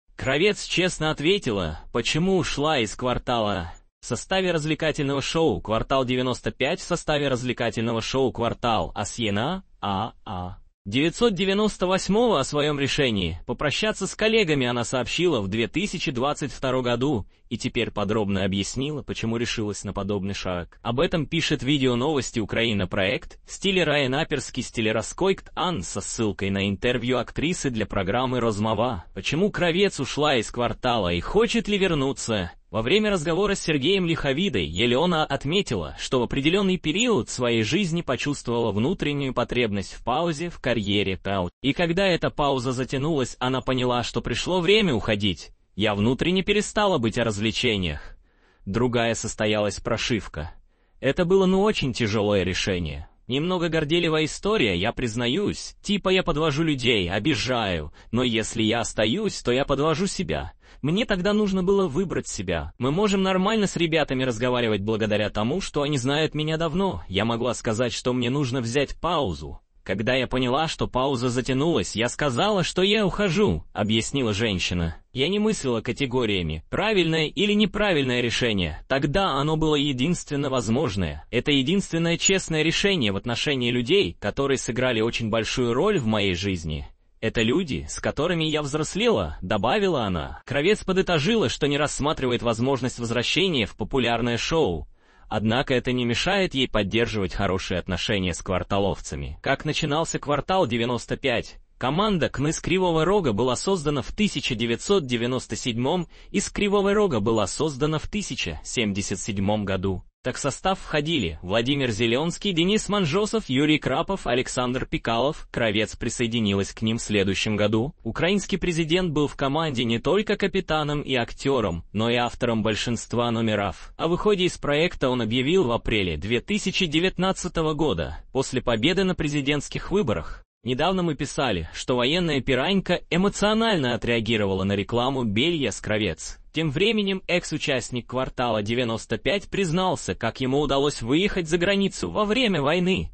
Кравец честно ответила, почему ушла из "Квартала" .
ЕленаКравец Актриса Звёзды Карьера В составе развлекательного шоу "Квартал 95" актриса Елена Кравец была около 25 лет - с 1998-го. О своем решении попрощаться с коллегами она сообщила в 2022 году, и теперь подробно объяснила, почему решилась на подобный шаг. Об этом пишет Видео новости Украина(проект Styler) со ссылкой на интервью актрисы для программы "Розмова". Почему Кравец ушла из "Квартала" и хочет ли вернуться.
Во время разговора с Сергеем Лиховидой Елена отметила, что в определенный период своей жизни почувствовала внутренню